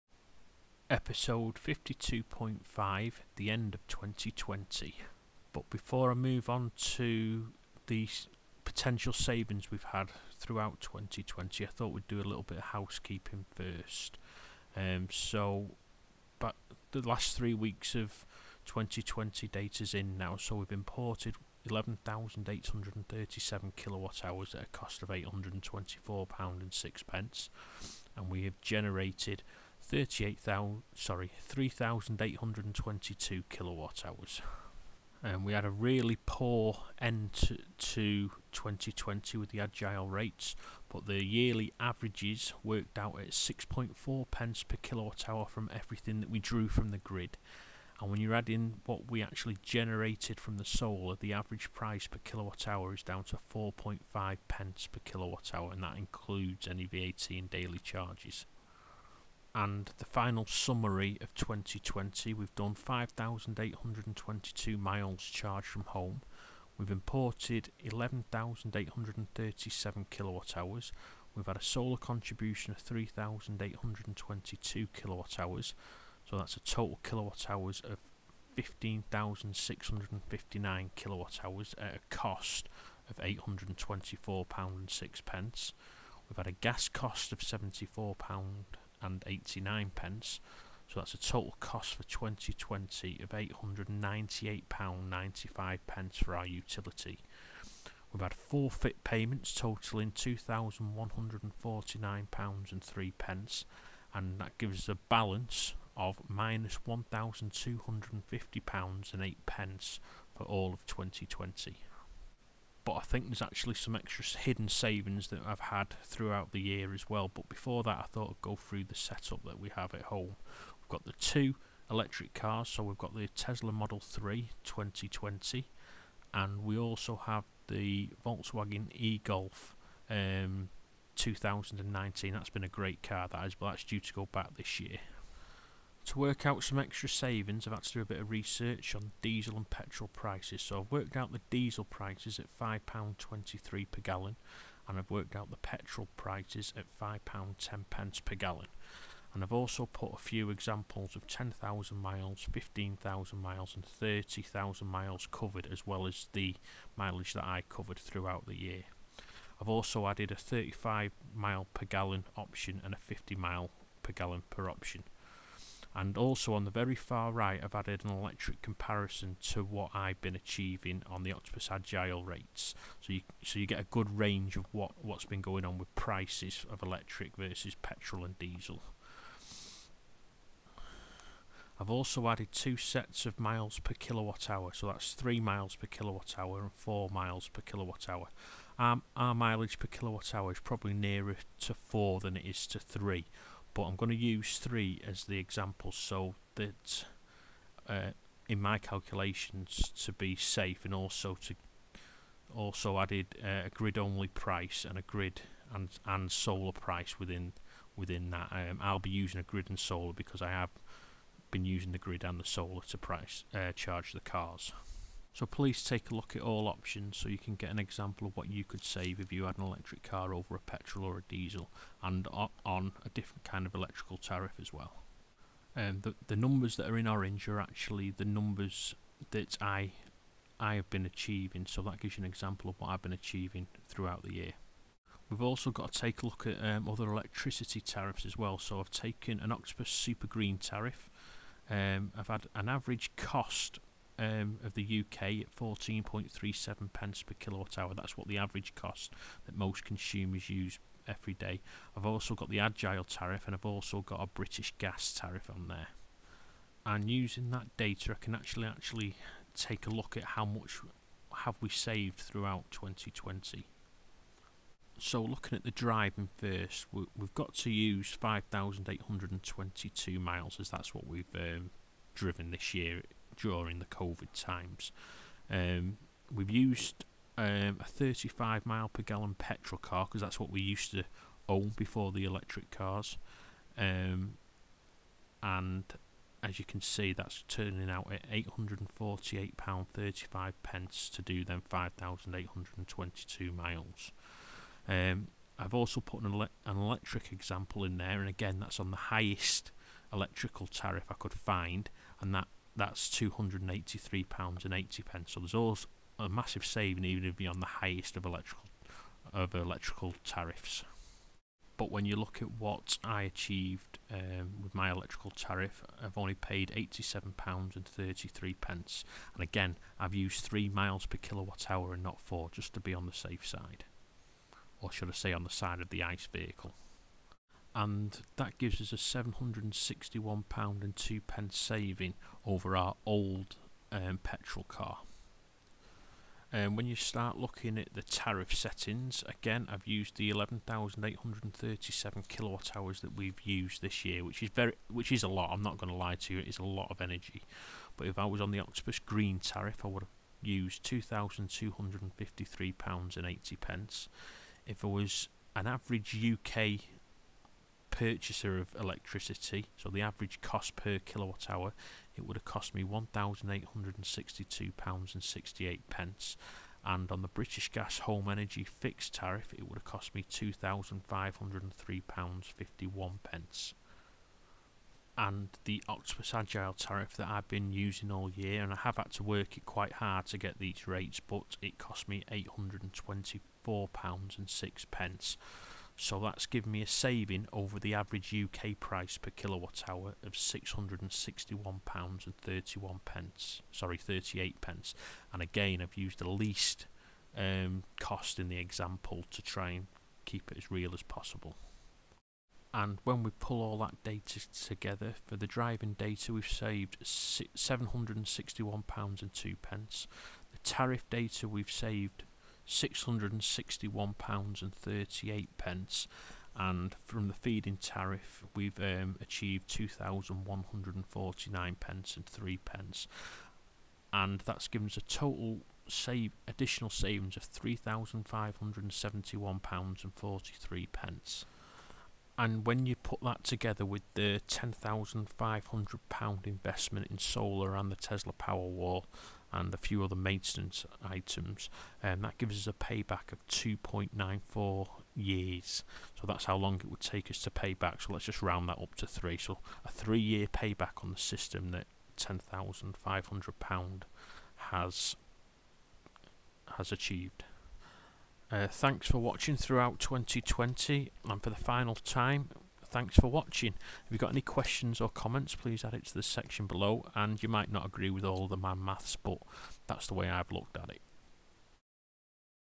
Final savings of 2020, on octopus agile with solar PV and the Tesla powerwall 2. MAN MATH
Its been a long year for more than one reason in 2020 but working the octopus agile tariff very hard and maximising the savings with the tesla powerwall and solar PV and is tariffs like this the future of energy.

Setup
4KwP solar array with a SMA 3000TL (3KW) inverter
Tesla powerwall 2 with gateway 2 (backup)
VW Egolf 38Kwh
12kw electric boiler & x2 3kw heating elements for water
My energi Eddi
Octopus Agile Tariff
ohme smart charge cable linked to the agile tariff.

other solar stats below from around the UK